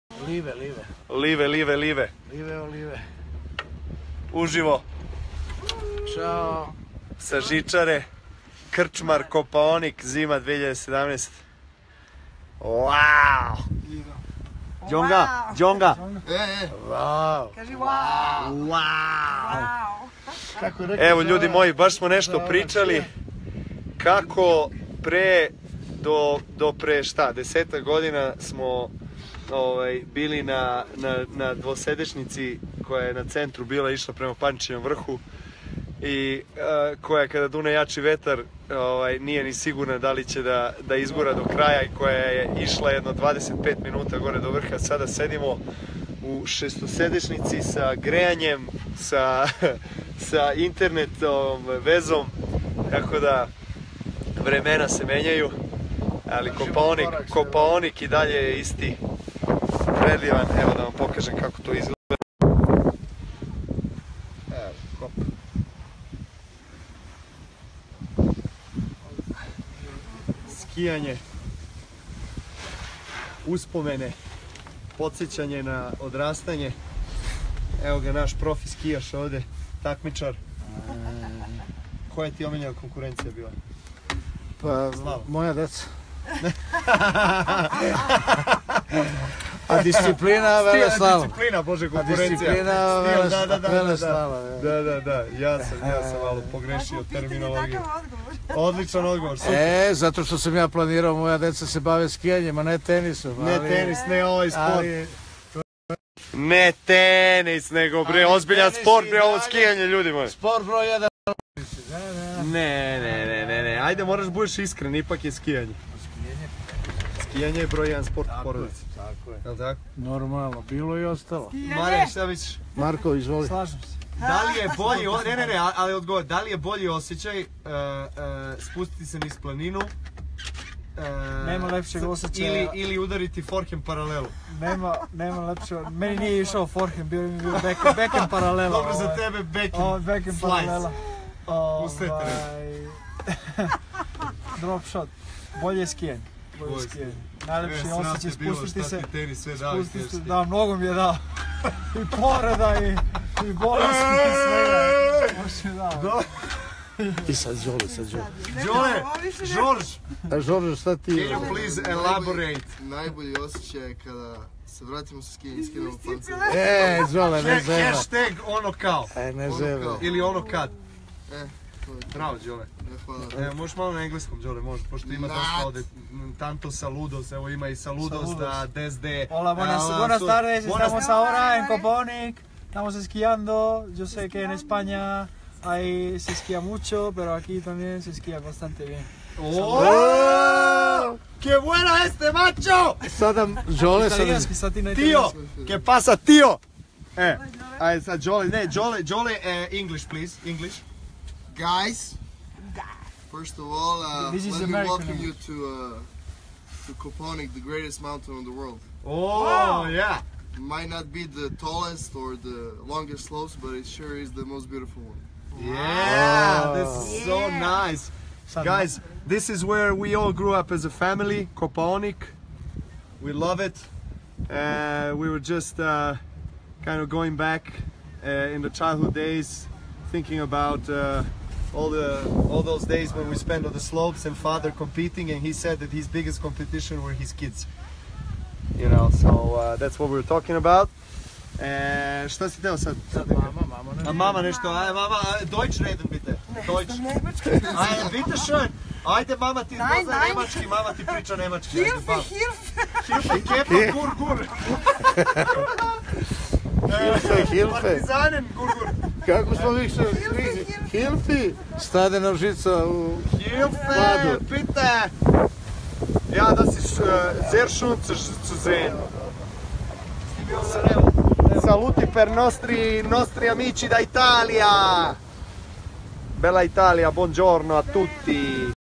Novak skiing with family in Kopaonik
Novak Djokovic skiing with family in Kopaonik (in Serbia)
source: Novak's facebook